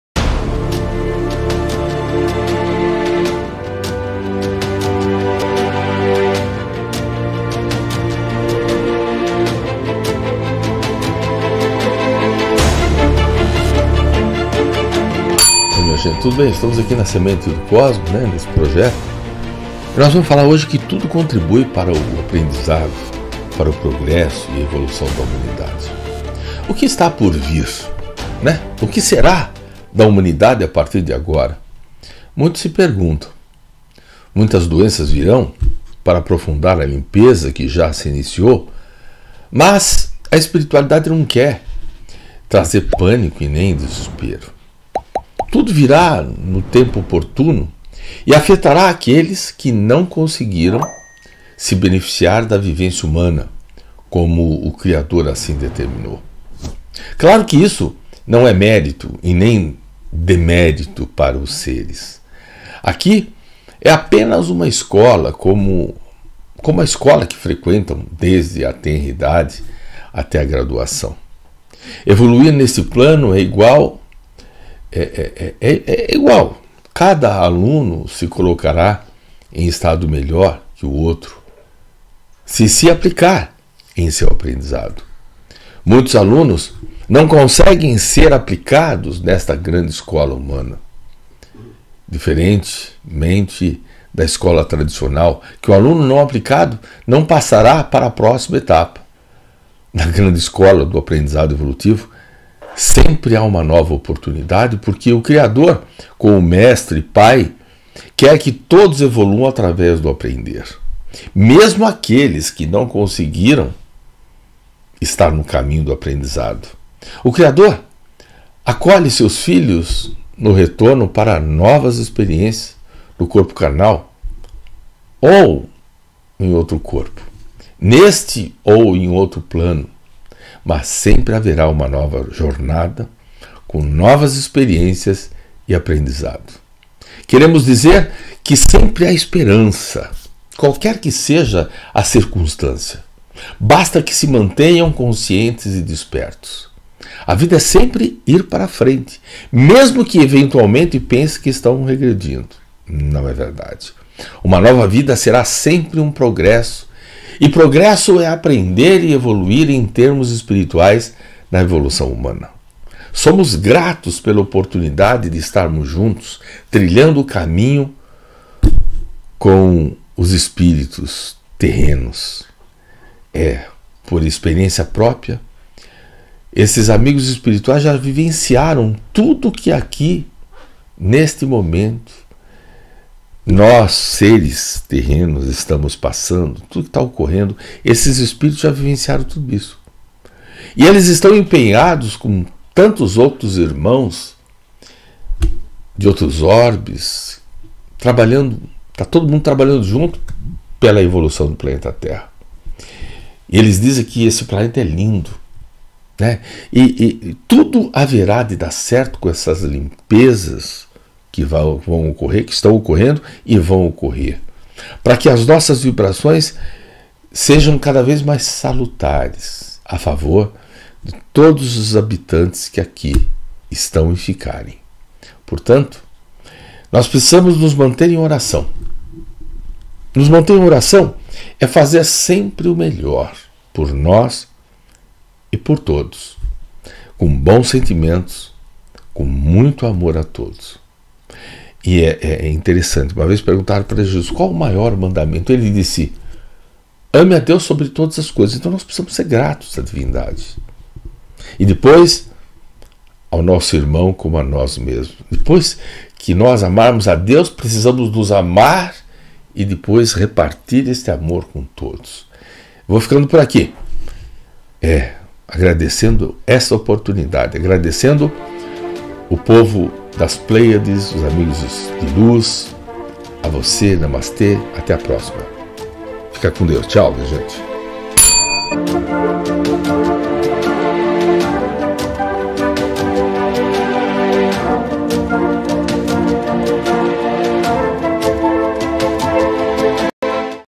Tudo Contribui Para o Aprendizado, Progresso e Evolução | Sinuhe da Silva Vieira-Sementes do Cosmo
Tudo virá no tempo oportuno e afetará aqueles que não conseguiram se beneficiar da convivência humana.
Veja esse vídeo! | Tudo Contribui Para o Aprendizado, Progresso e Evolução | Sinuhe da Silva Vieira - Sementes do Cosmo, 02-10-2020

Lembre-se de "curtir" e ""compartilhar" com os seus amigos.
Agradecemos.